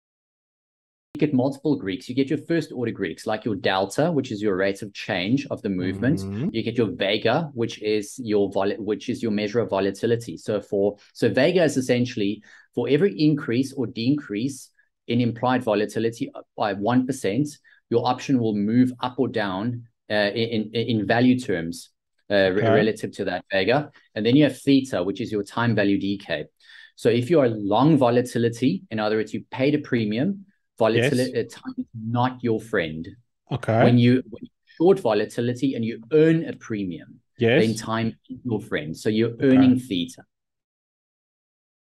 Trading Options : An introduction and getting to know the Greeks | Adrian Alberts | SGM 72
You might have heard about trading Options and the phrases calls or puts and strikes being used, but what does it all mean?

I'm joined by a friend of the show, Adrian Alberts to give listeners an introduction to the Greeks!

We also take a look at the current state of the market in this interesting and educational episode

Adrian is an accomplished public speaker, trader and Podcast host